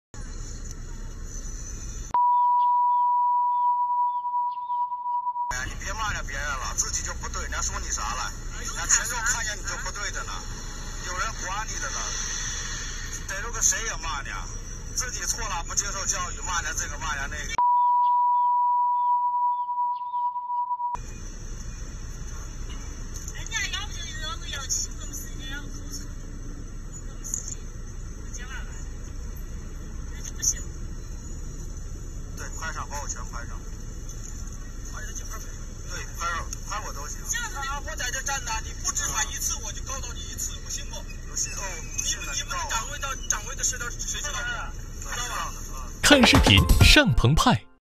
女子骑车闯红灯不听劝，喊来丈夫骂交警
9月4日，内蒙古呼和浩特。一女子骑电动车闯红灯，无视交警哨声仍继续行驶。面对批评教育，女子接连辱骂交警，甚至喊来丈夫一同辱骂。